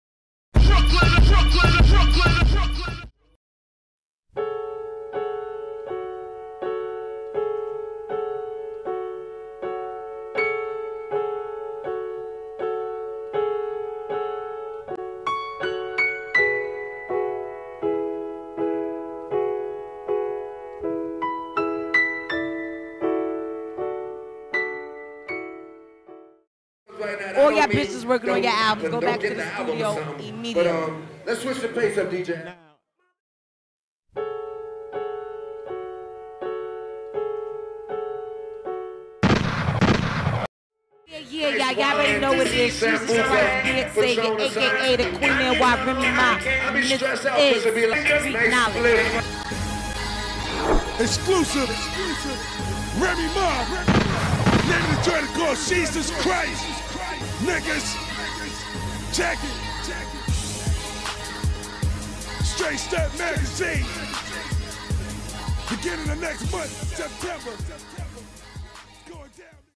REMY MA ETHERS ALL FEMALE RAPPERS(EXCLUSIVE!!!!!)
THE WORLD ACCORDING TO REMY!!REMY MA SPEAKS ON FEMALE RAPPERS. HEAR HER TAKE ON OTHER FEMALES WHO ARE GETTING READY TO PUT OUT NEW MUSIC!! STREET KNOWLEDGE TV!!